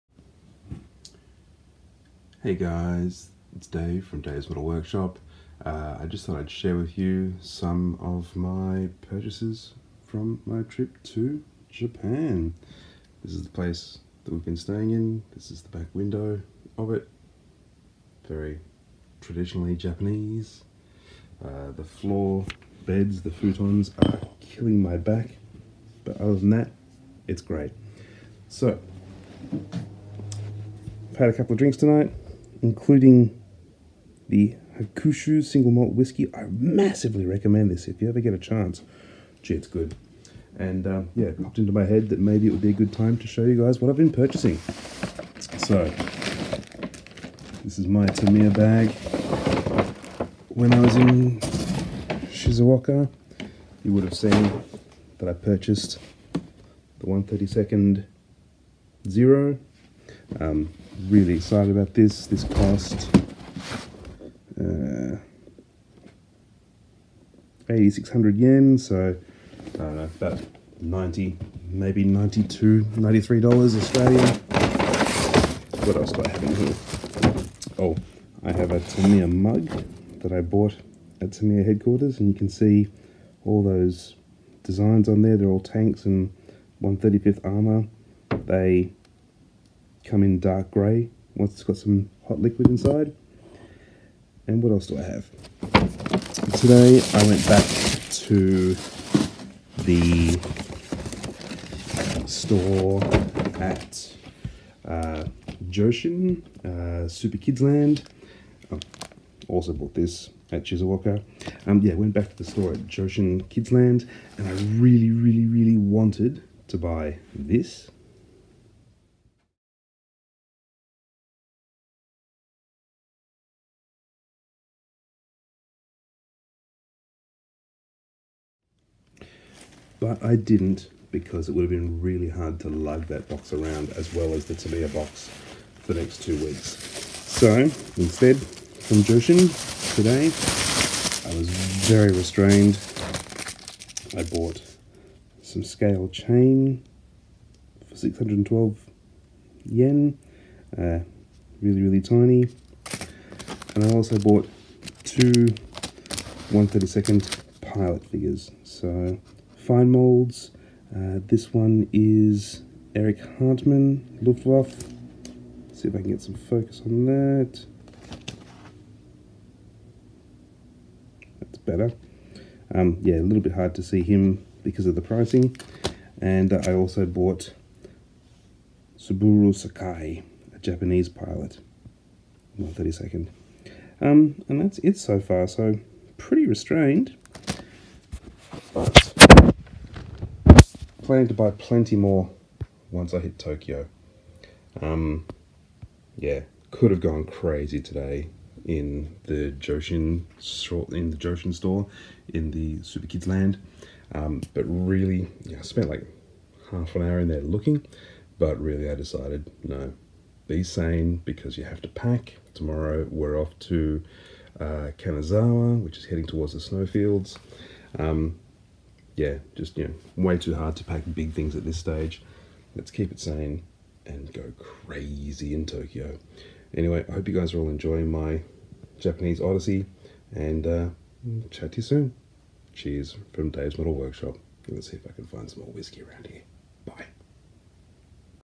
My Scale Model Purchases So Far In Japan
I’ve been pretty restrained so far with purchases of scale models here in Japan, mostly because I haven’t yet bought a third suitcase so anything I buy now I have to lug around the country until we get to our final destination, Tokyo. Once we hit Tokyo, it is going to be crazy time, I promise you.